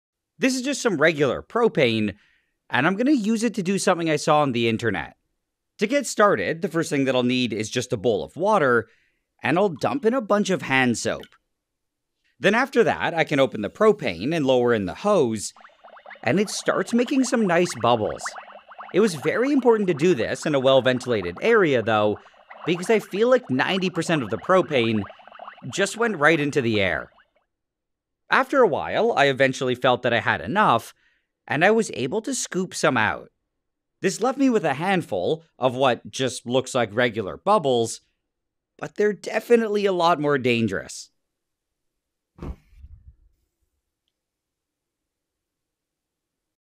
Making propane bubbles
This is just some regular propane, and I'm gonna use it to do something I saw on the internet.

WARNING: Propane is extremely flammable and potentially explosive. The experiment was performed in a controlled environment. Don't recreate anything you see.